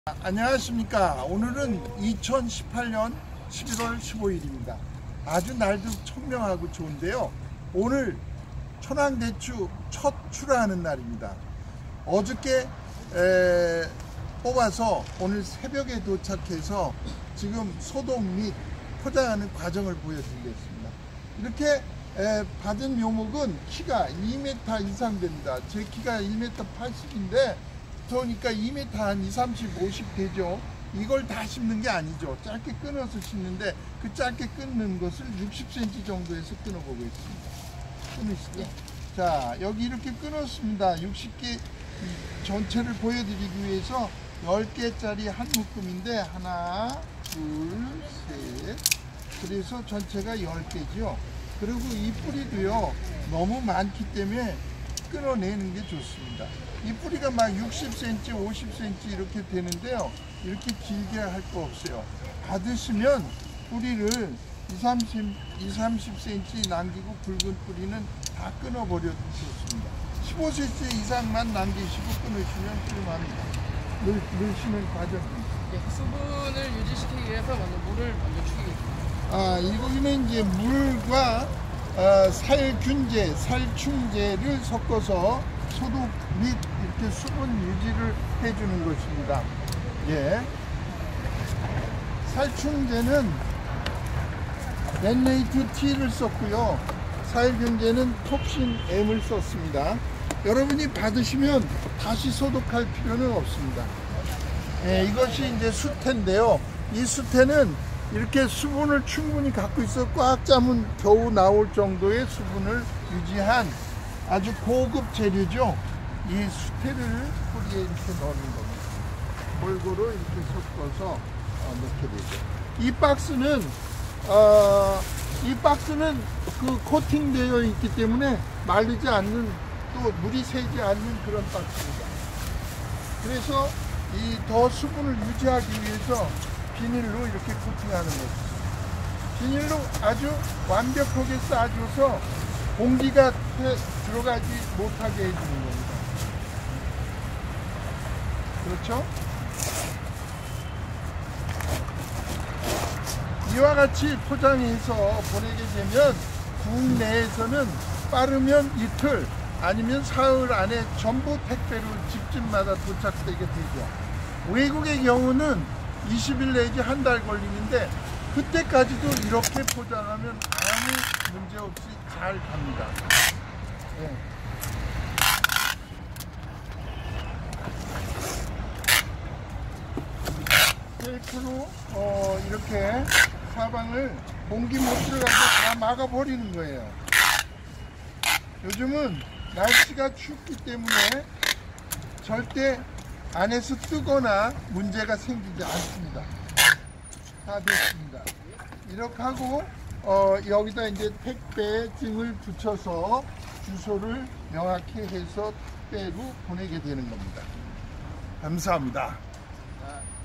::: 미림 Tube ::: 미림천황대추 묘목 택배포장하는 영상입니다
궁금하신 사항은 미림원예종묘 본사에 연락주시면 상담해 드리겠습니다